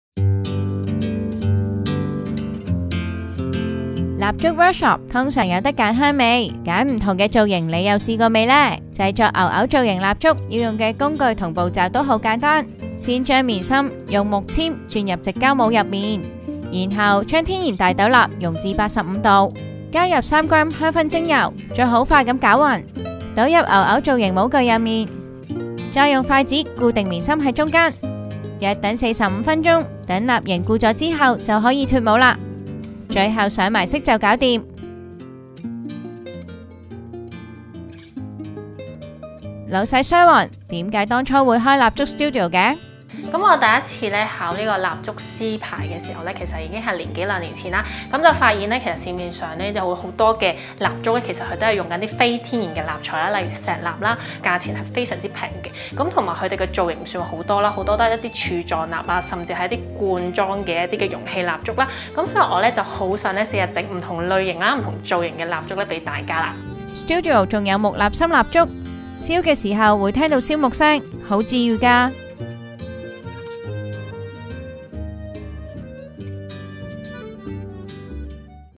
【蠟燭workshop】簡易DIY學堂 可愛造型蠟燭
蠟燭workshop試過唔少，通常以揀不同香氣為主。新開業的Shadle Studio，有雪山、水晶礦石、植物等超過20款造型蠟燭，仲可以客製化，如蛋糕造型、新年鐵罐蠟燭等。

【INFO】
Shadle Studio
地址：觀塘開聯工業中心B座12樓1201G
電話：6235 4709
時間：約 2 小時
價錢：每位399港元起
全方位緊貼《U Magazine》社交平台